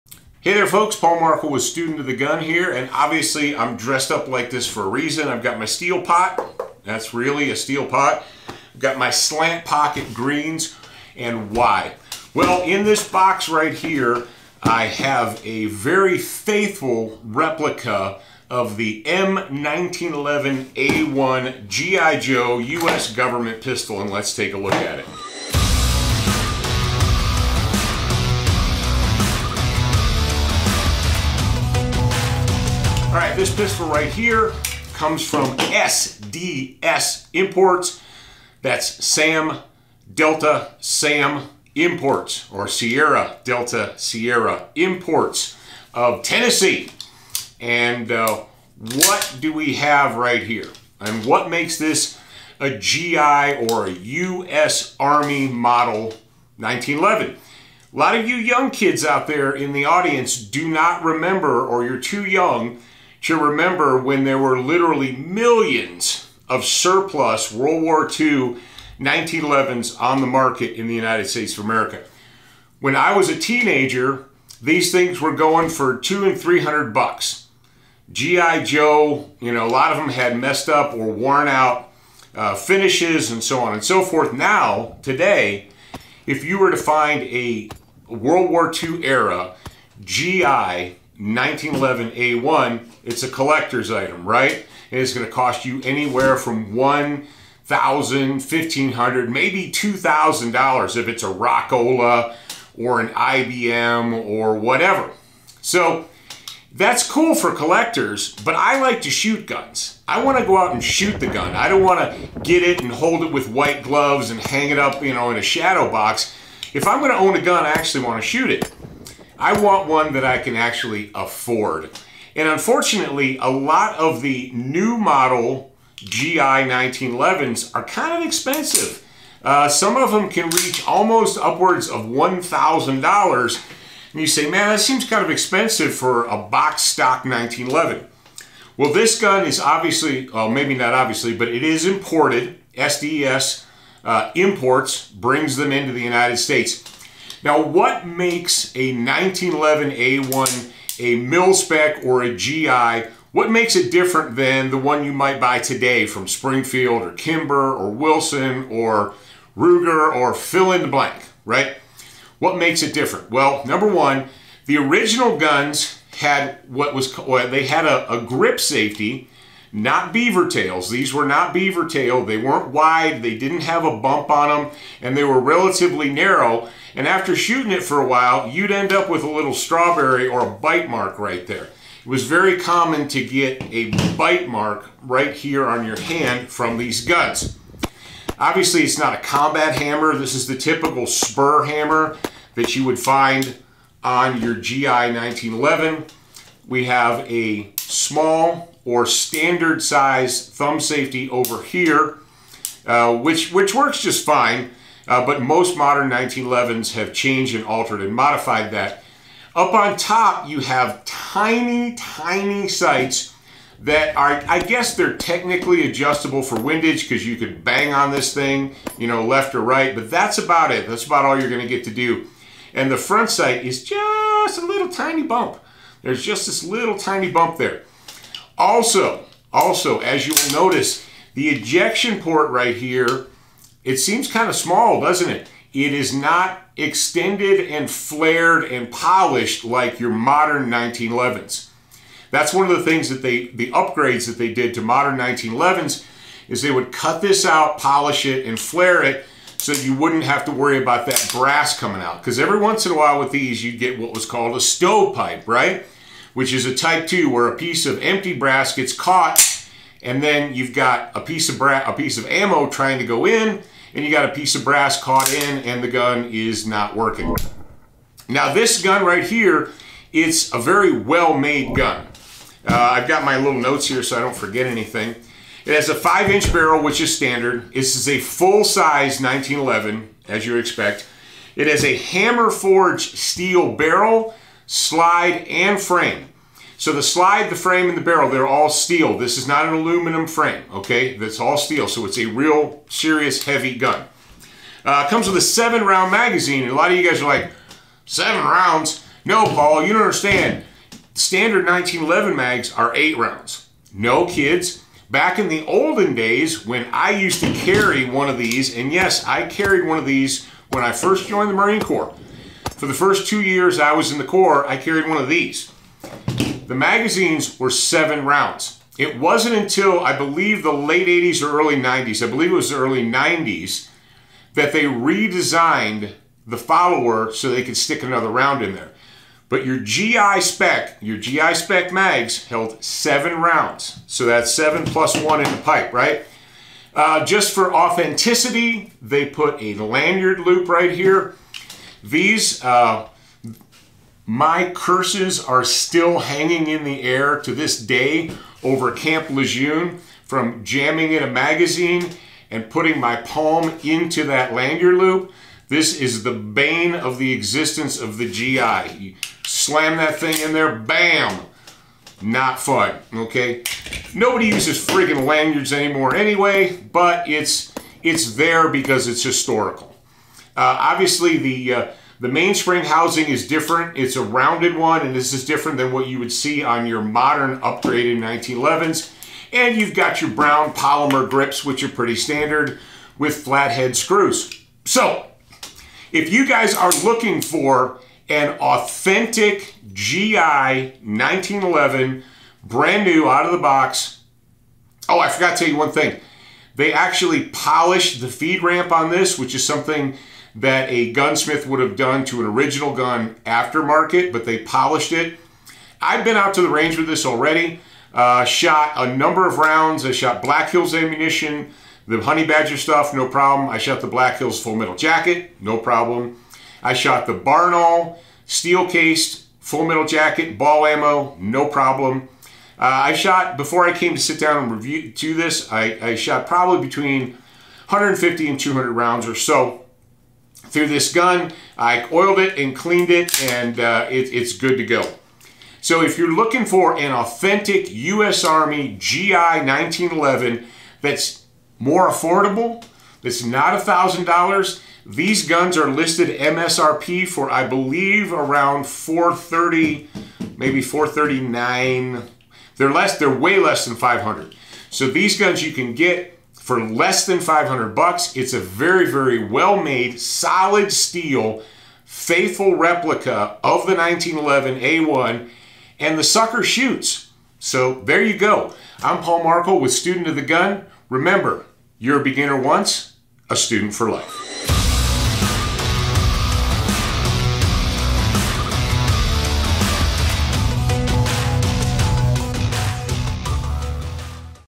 M1911 from SDS Imports
Ask your father or grandfather and they will tell you how quickly a generation passes away. When I was a young man, every gun person worth their salt knew and understood the history of John Brownings, M1911 .45 ACP pistol. Keep in mind, we did not have an internet, we had to read books and magazines.

As my words hit the paper here, it has been 35 years since the US Army decided to replace the M1911A1 pistol with the M9 Beretta. For those too young to remember, there was much consternation and contention over that choice. Nonetheless, the die was cast and over a period of about five years, the US Armed Forces made the change over. It was mid-1989 before the unit where I was assigned made the switch.